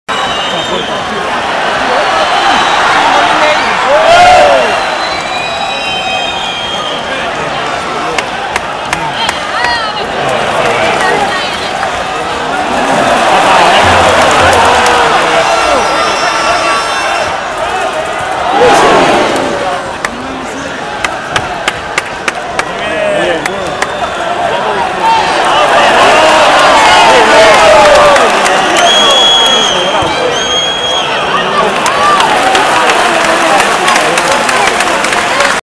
SD Eibar 1 - 0 Real Oviedo, en Ipurua, 15 Junio 2013
SD Eibar 1 - 0 Real Oviedo en Ipurua, 15 Junio 2013, gran ambiente y victoria de los armeros, en la ida 1-2, pasa el Eibar a la última eliminatoria fase ascenso a 2ª división, Aupa Eibar. gran afición oviedista y armera.
.CE L'Hospitalet es el último rival para subir a 2ª.